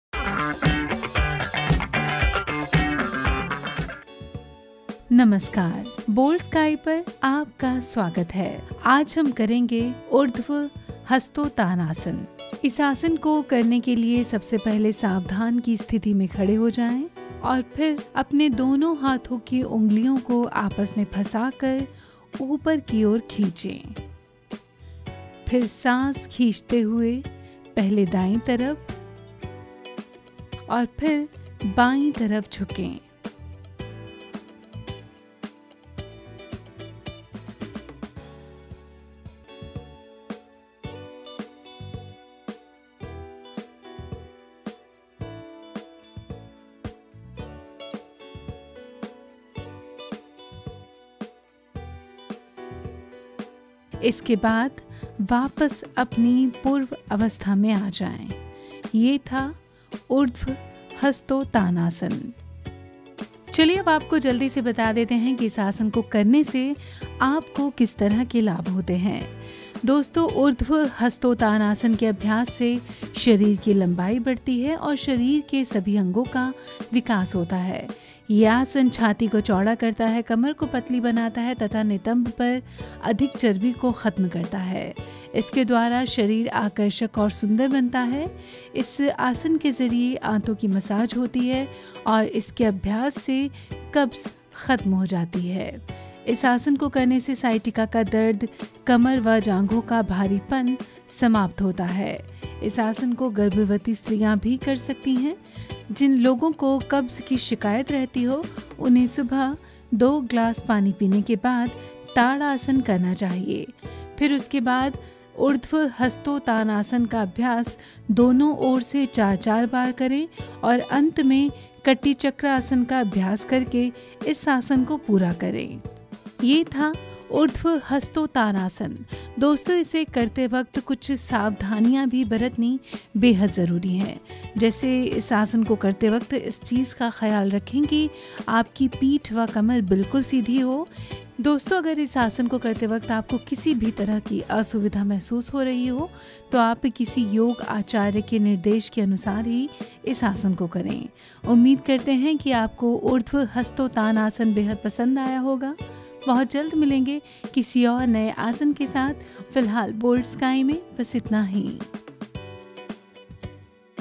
How to do Urdhva Hastotasana, ऊर्ध्व हस्तोतानासन | पुराने से पुराने कब्ज़ को ख़त्म करता है ये आसन
Let's learn about Urdhva Hastotasana, ऊर्ध्व हस्तोतानासन in today's video. Consciously, or subconsciously, when you roll out of bed every morning, you raise your arms, stretch as your spine arches, and your head hangs at the back. It energizes you after you have had a full night’s sleep. That stretching is what this asana is all about. Urdhva Hastotasana is a part of the Surya Namaskar sequence. It is usually a short asana, done for a short duration. Watch the tutorial here.
ऊर्ध्व हस्तोतानासन के अभ्यास से शरीर की लंबाई बढ़ती है और शरीर के सभी अंगो का विकास होता है | ये आसन छाती को चौड़ा करता है,कमर को पतली बनाता है तथा नितम्ब पर जमी अतिरिक्त चर्बी को ख़त्म करता है,इस आसन के अभ्यास से आंतों की मसाज होती है जिससे पुरानी से पुरानी कब्ज़ ख़त्म होती है,आइये जाने ये आसन किस तरह किया जाता है.